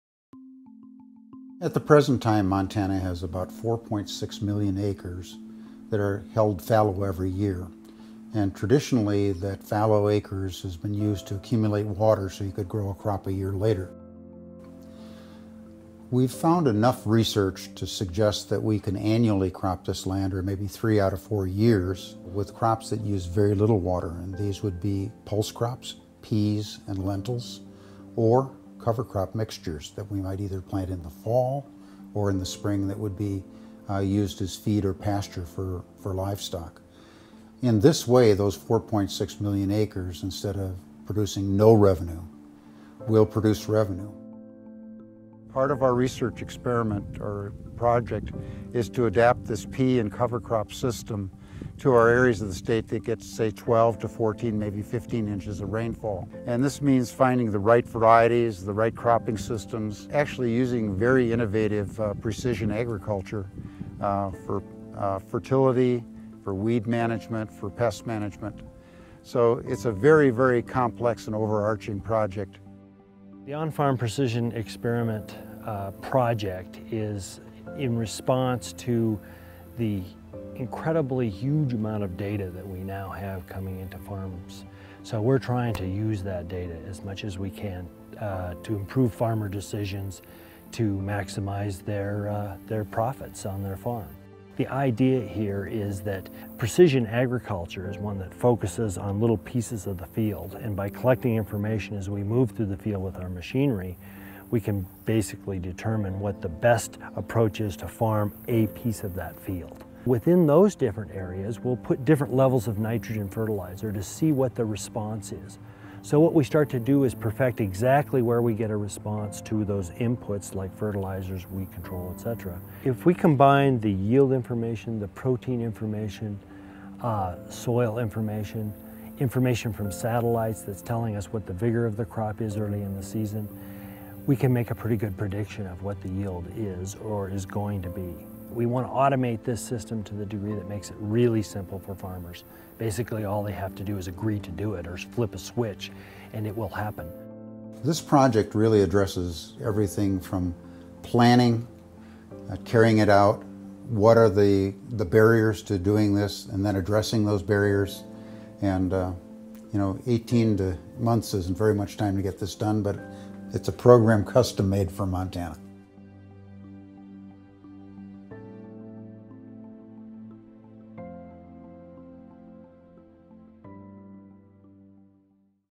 Improving Revenues & Efficiency of Montana's Farm & Ranch Lands
Thanks in part to a grant from the Montana Research and Economic Development Initiative, Montana State Researchers are looking at ways to improve efficiency and revenues on Montana's farm and ranch lands by utilizing pulse and cover crops as well as precision agriculture techniques.